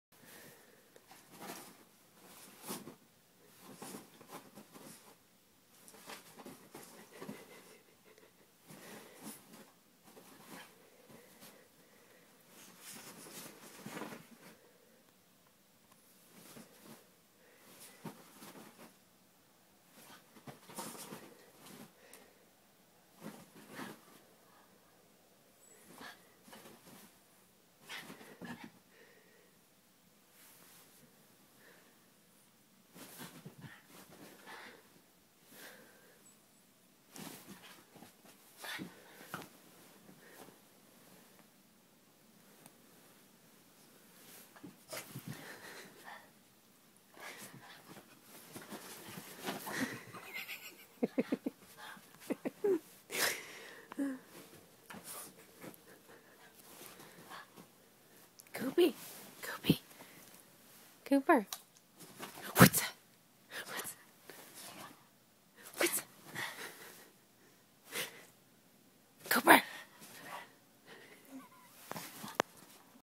Worlds most terrified dog!!!!
Funny video of my dog scared of a spray bottle.